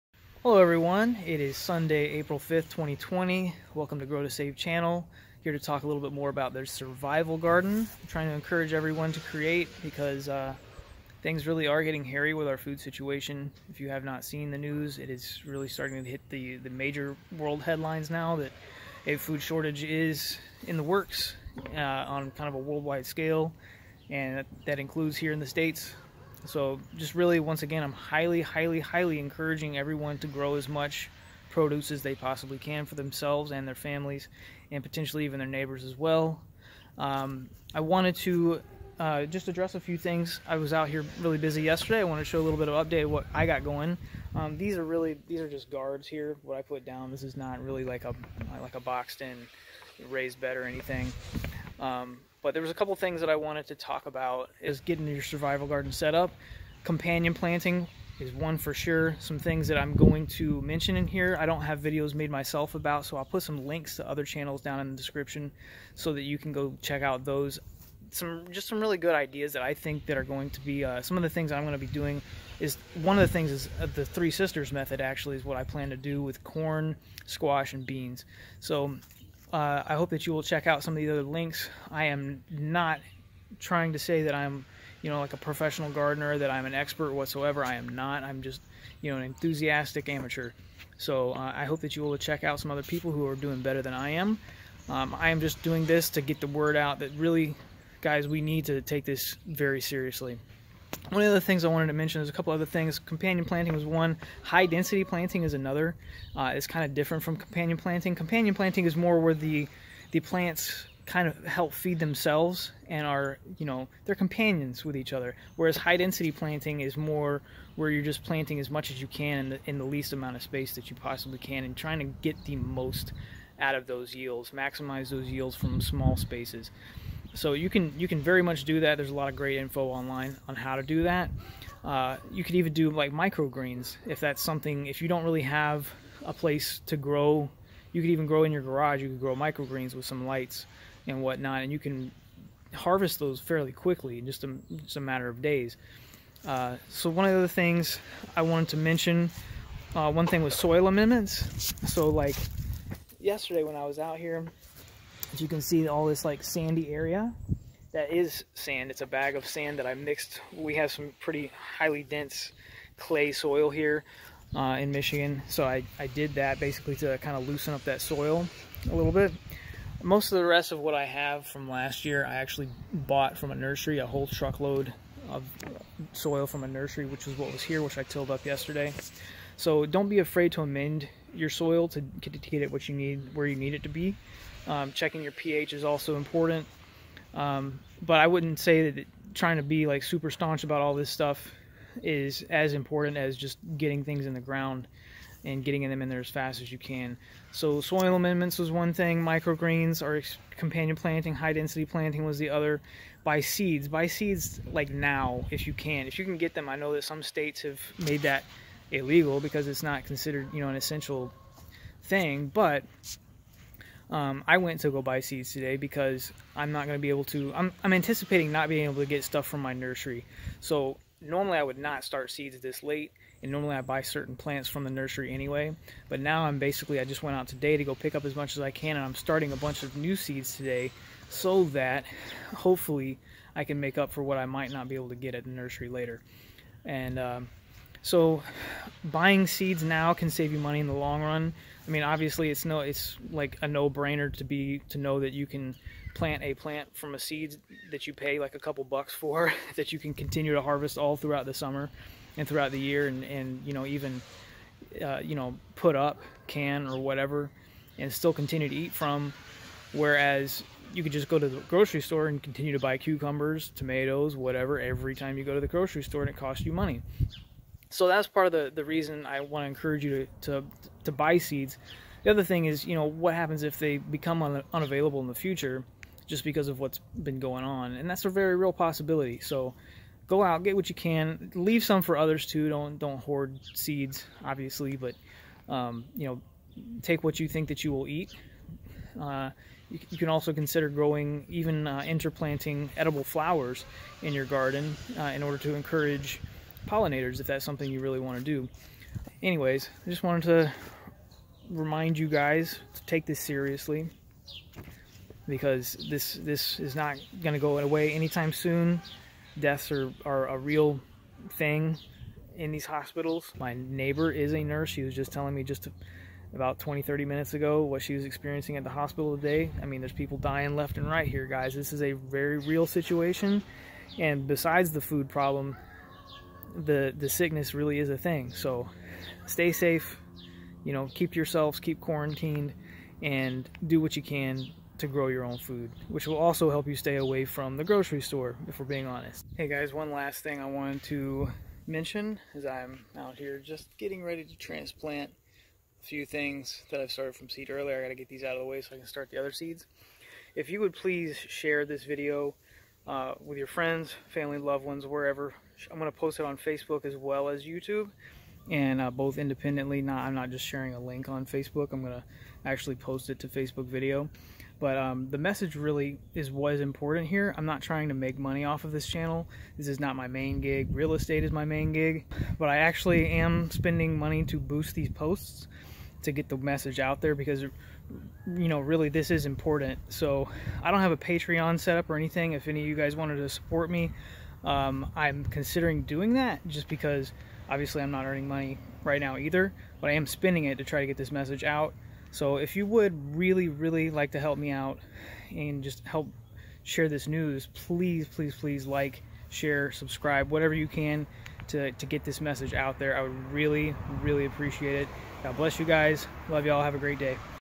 Survival Garden Tips Spring 2020. Ideas to get growing during the crisis.
Hey folks! This video covers a few tips and ideas to get a food source of your own growing this year before we experience shortages at the grocery stores.

Here are some links to other videos that I mentioned:

Grow 500% MORE Vegetables in 5 Times LESS Space!

Here's a link to an article I wrote a while back on soil amendments: